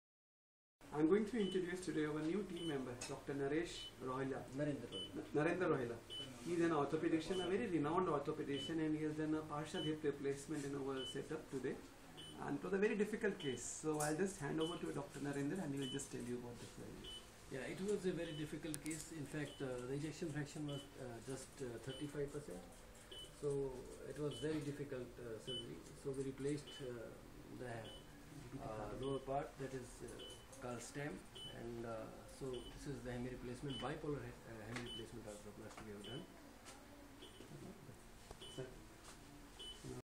Interview of Dr Narender, Orthopaedician .-Care Well Medical Centre
Interview of Dr Narender, Orthopaedician who conducted Bipolar hip replacement in  a critical patient with ejection fraction  of heart only 35%. Patient was rejected in many hospitals. The surgery was successfully conducted at very economical price at Care well medical Centre,

Thanks & Regards
Dr.Sandeep Bhasin
Sr. Cosmetic Surgeon